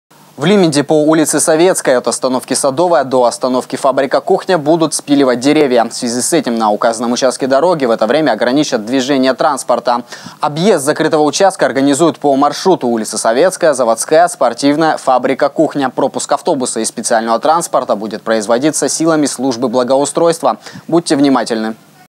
Валка насаждений 18 09 2017
В течение нескольких дней – с 19 по 21 сентября с половины десятого утра до пяти часов вечера в микрорайоне Лименда планируется проведение работ по благоустройству.